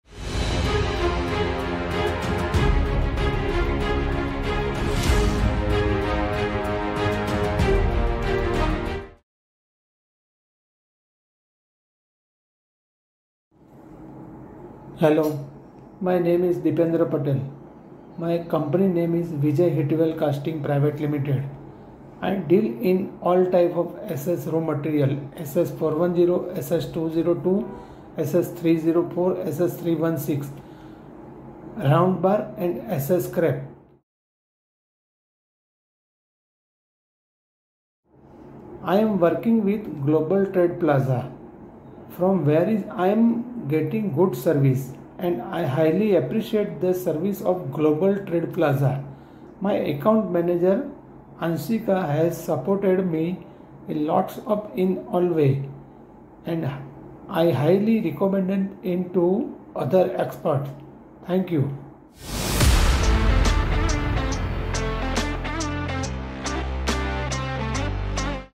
Experience Of Our Client With GTP!! | Vijay Heatwell Casting PVT Ltd.
Mr. Jayesh Kumar V Shah, the owner of Vijay Heatwell Casting PVT Ltd. from India explained his service experience at GTP and mentioned his sales experience by saying "Global Trade Plaza provides excellent service, which I greatly appreciate. My account manager also provided me with extensive support in all areas, and I highly recommend GTP to other businesses." We are grateful for such a great review of our services and commit to becoming more efficient in the future for more better supplies.

𝗝𝗼𝗶𝗻 𝗚𝗹𝗼𝗯𝗮𝗹 𝗧𝗿𝗮𝗱𝗲 𝗣𝗹𝗮𝘇𝗮 𝘁𝗼𝗱𝗮𝘆 𝗼𝗿 𝗴𝗶𝘃𝗲 𝘂𝘀 𝗮 𝗰𝗮𝗹𝗹 𝗮𝘁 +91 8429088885.